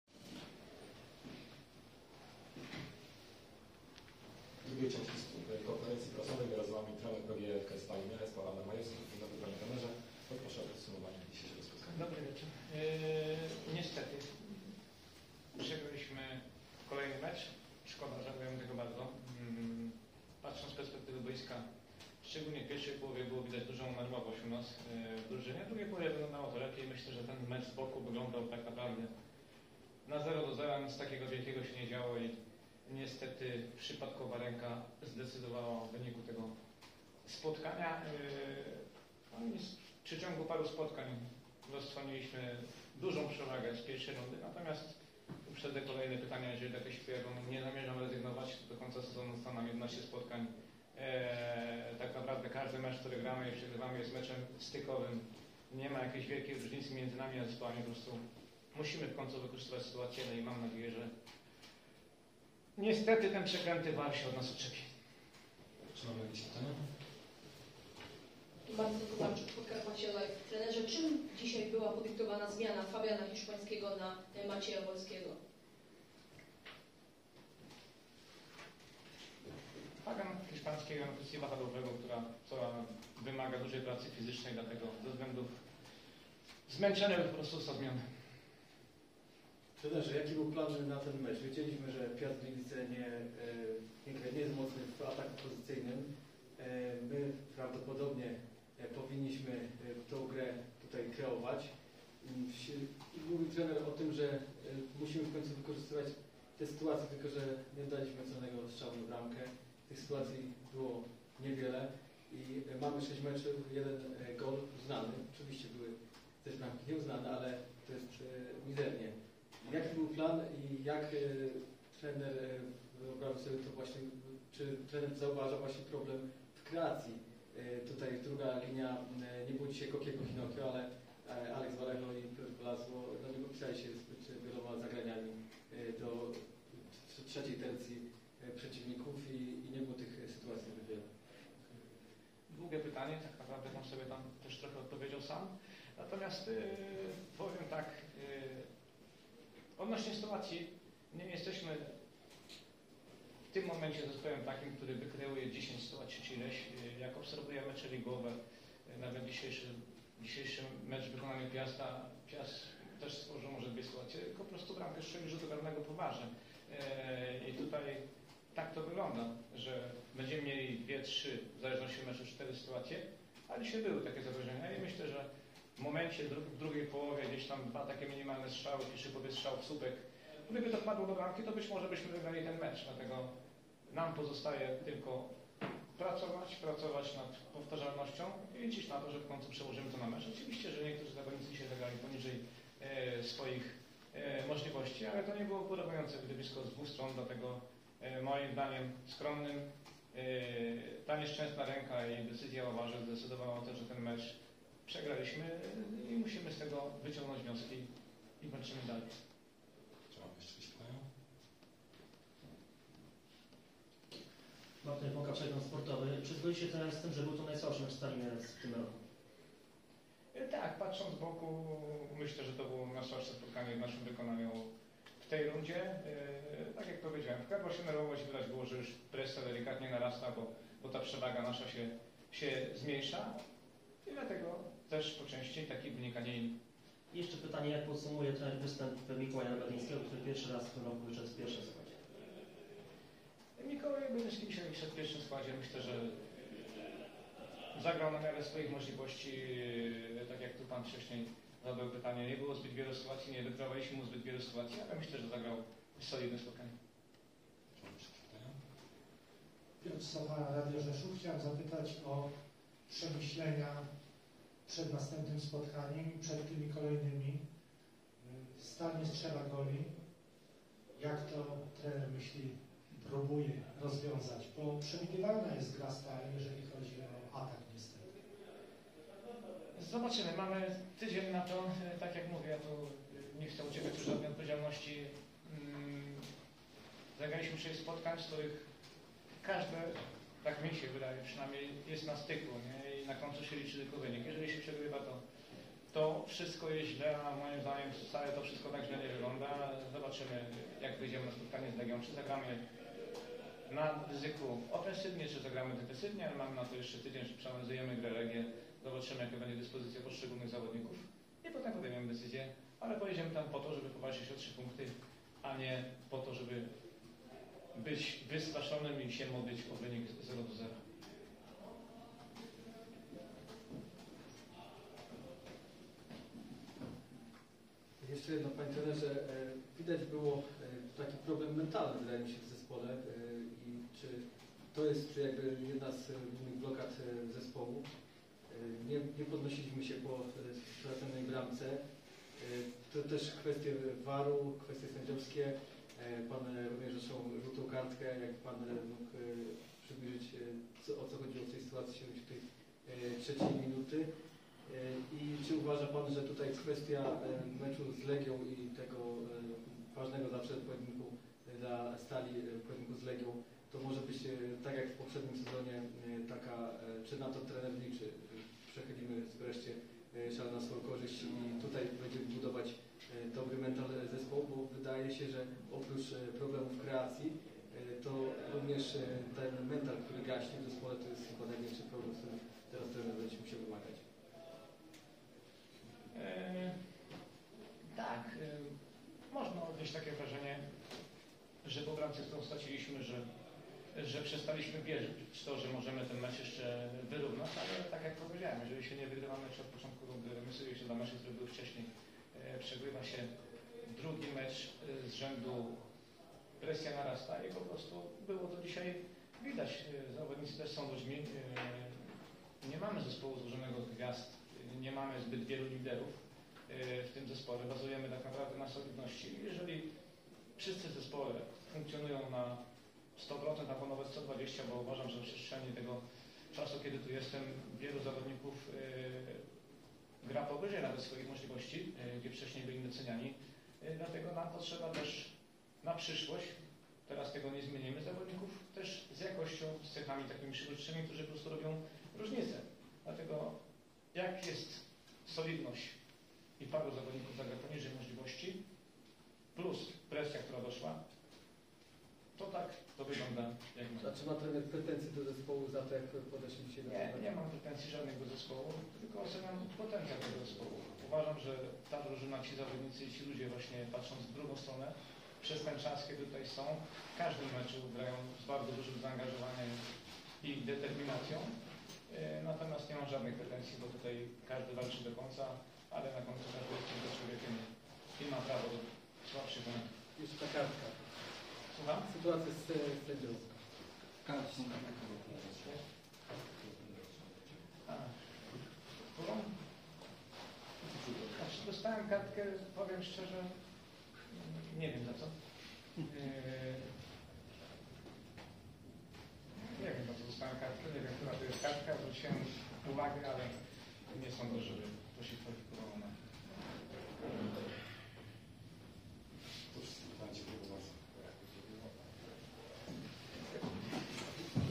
Wypowiedź trenera Stali Mielec Adama Majewskiego po meczu z Piastem Gliwice
Trener Stali Mielec Adam Majewski po przegranym meczu ekstraklasy z Piastem Gliwice.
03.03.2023 Stal Mielec - Piast Gliwice 0:2